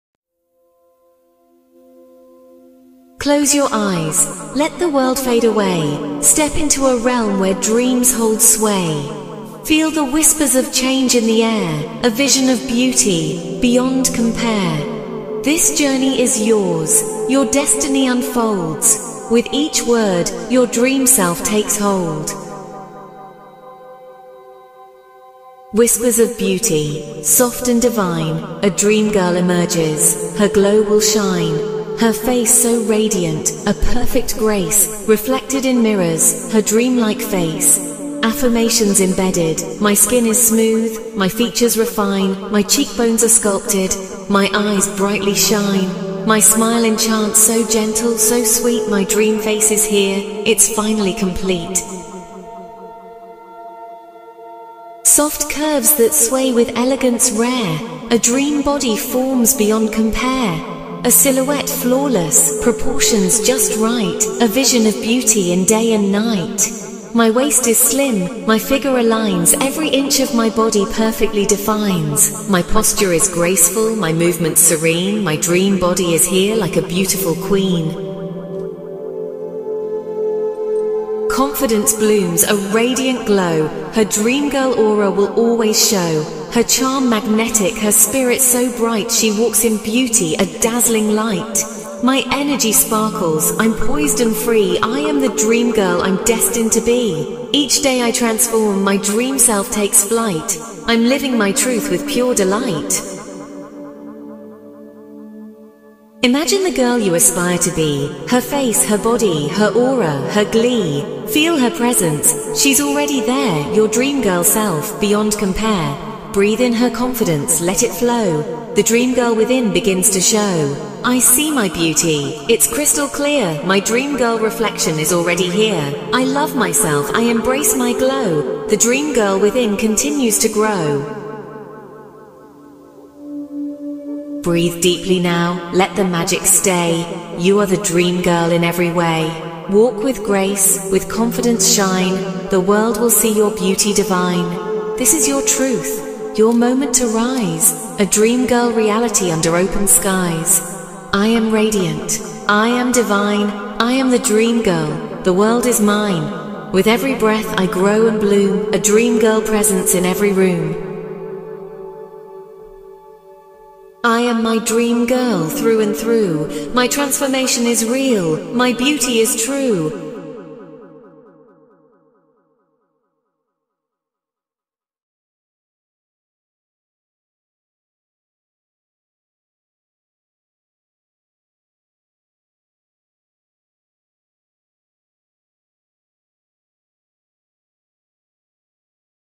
🌸 Transform Into Your Dream Girl 🌸 | MTF Subliminal for Dream Face, Dream Body & Dream Aura
Unleash your inner dream girl with this powerful subliminal designed to help you manifest your dream face, body, and aura effortlessly. 🌺

This subliminal features:
✨ Over 1000 positive affirmations woven into poetic verses
✨ A calming and immersive visualization journey
✨ Affirmations for beauty, confidence, and elegance

Listen daily and allow your transformation to unfold naturally. This video is crafted with love and precision to guide you toward embracing your ultimate dream self. 💖

Instructions:
🎧 Use headphones for maximum effect.
💆‍♀️ Relax, close your eyes, and visualize your dream self.
🕊 Listen as often as you desire to reinforce the changes.

Remember, your transformation starts from within. Believe in your journey and trust the process. You are already becoming your dream girl. 💕